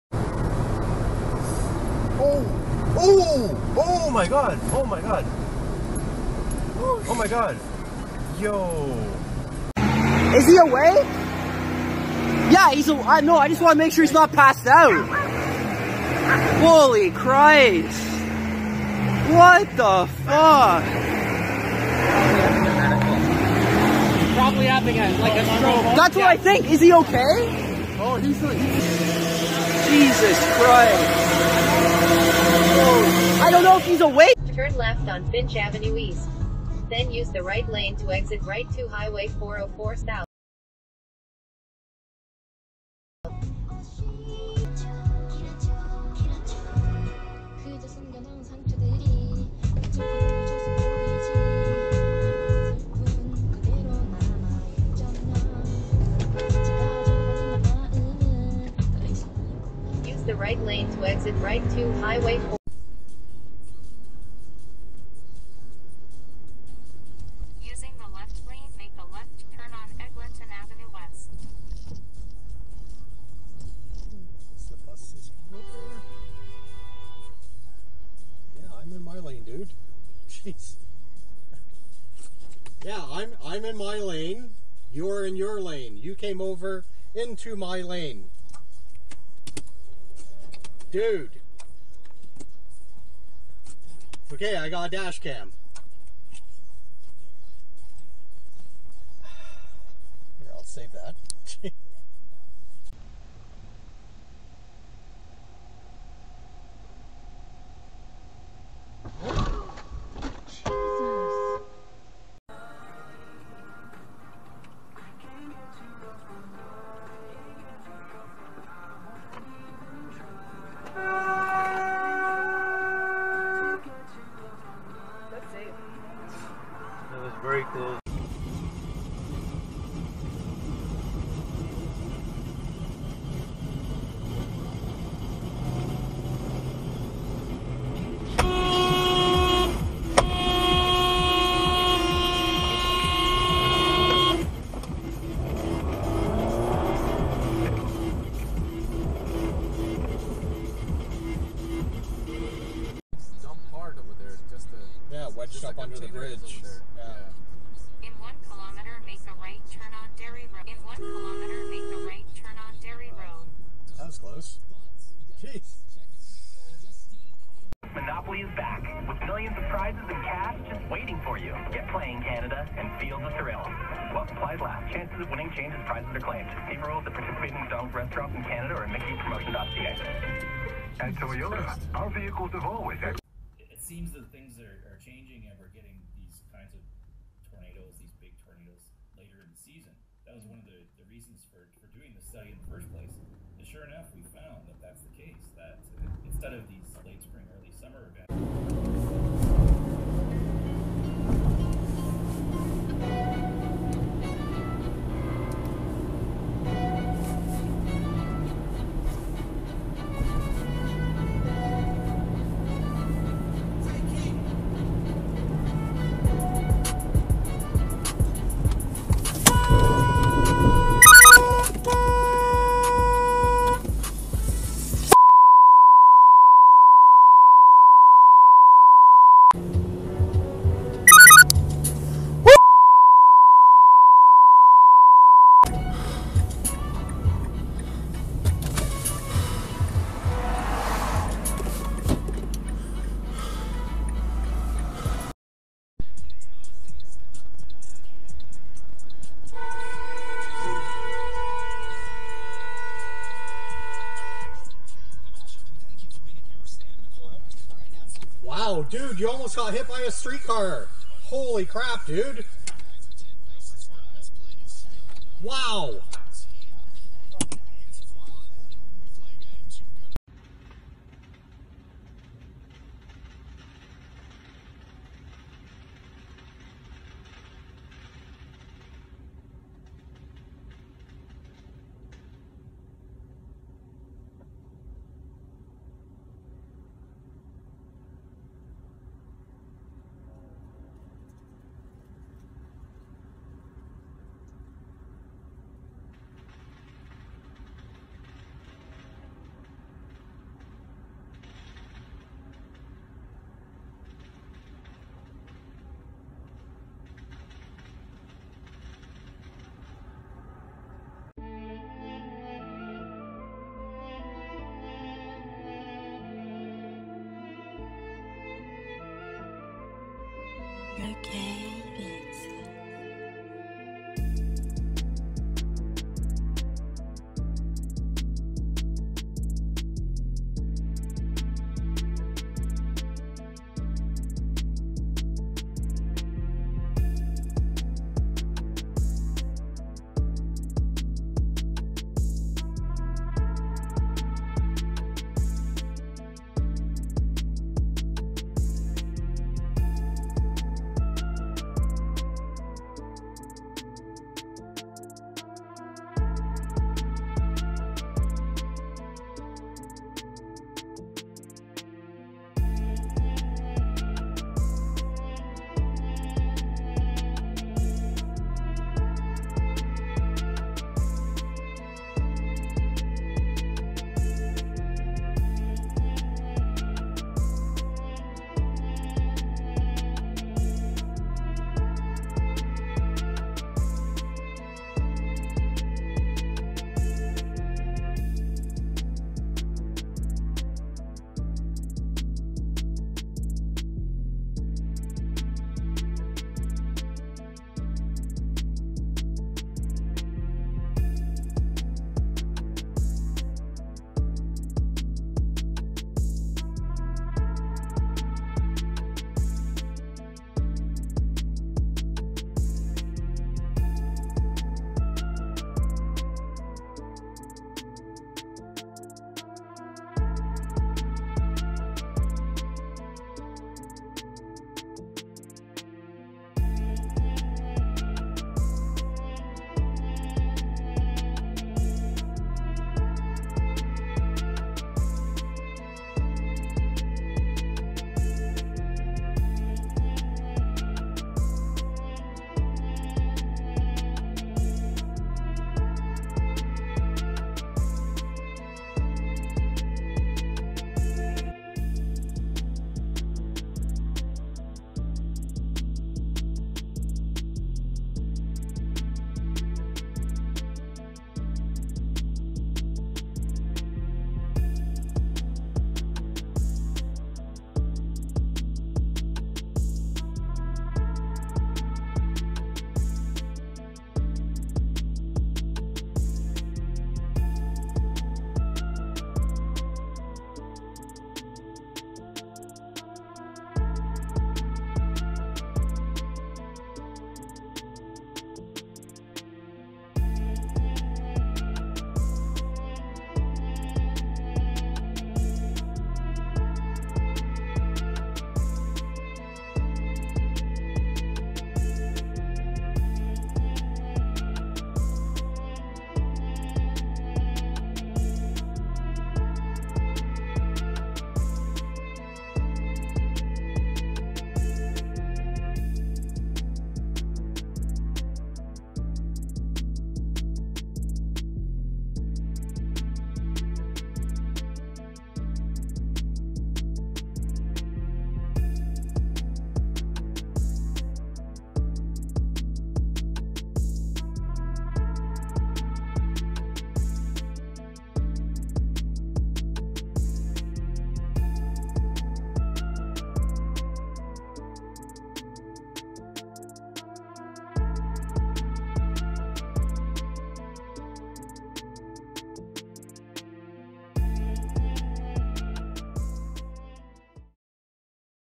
Bad Toronto Drivers Vol. 1 - Crash on 401, Cops Catch Road Rage, TTC Scares and More
In the first instalment of Toronto Bad Drivers, we see a few crashes, some road rage, and a few interactions with the TTC. Who's worse, Toronto or Calgarydrivers?